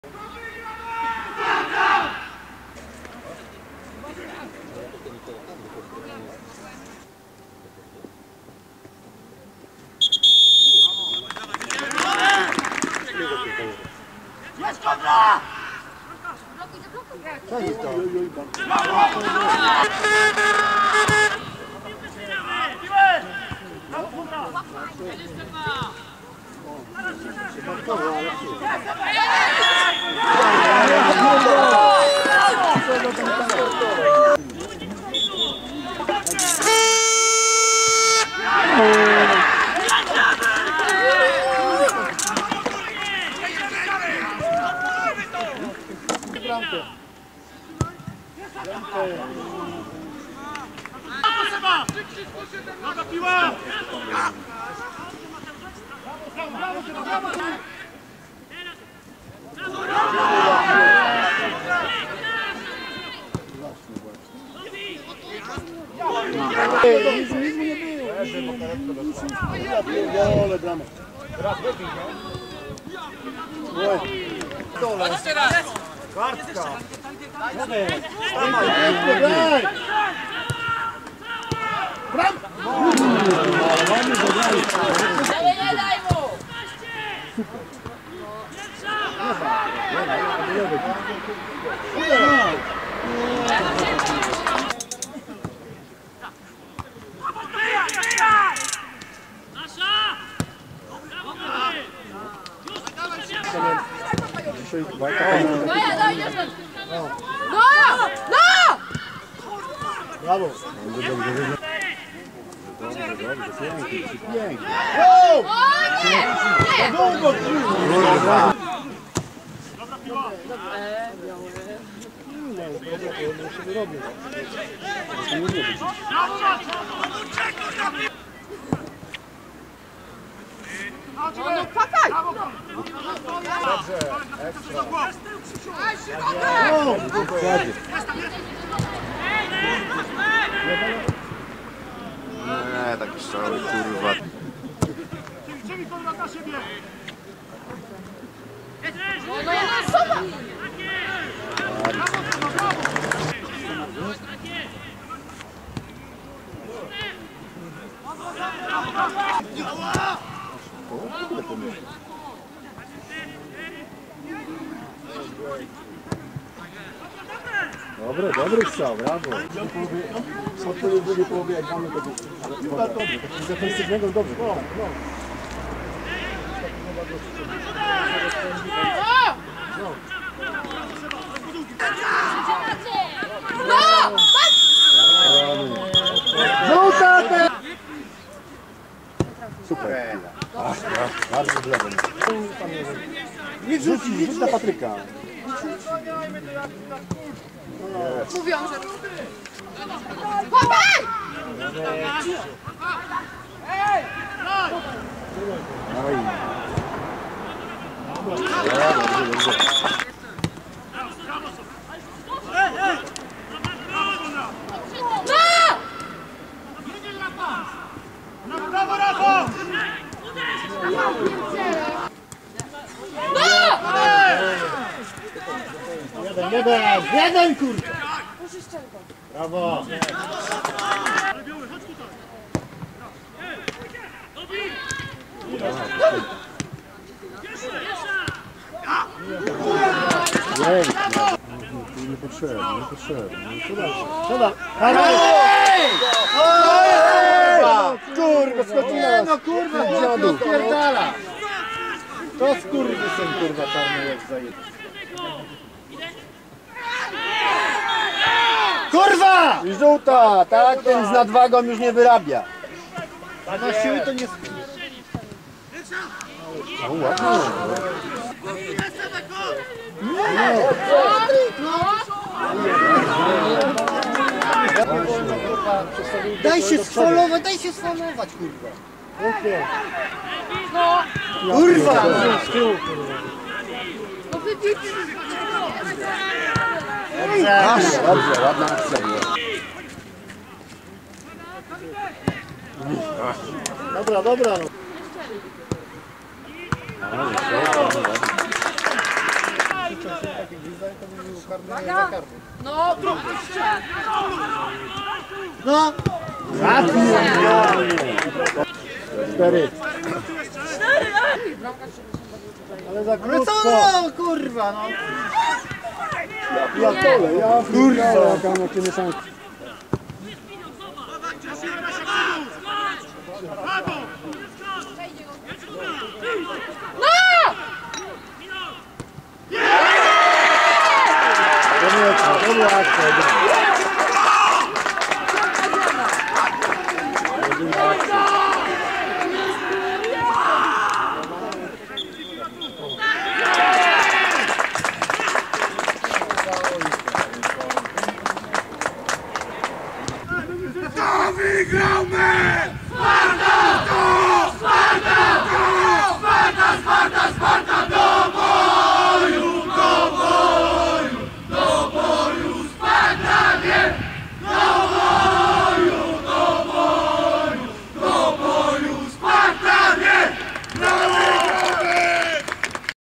LKS Kamienica Polska - Sparta Szczekociny (22 X 2016)
LKS Kamienica Polska - Sparta Szczekociny 0:2 (0:1)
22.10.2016
A Klasa Częstochowa II
sezon 2016/17
Mecz 10. kolejki
piłka nożna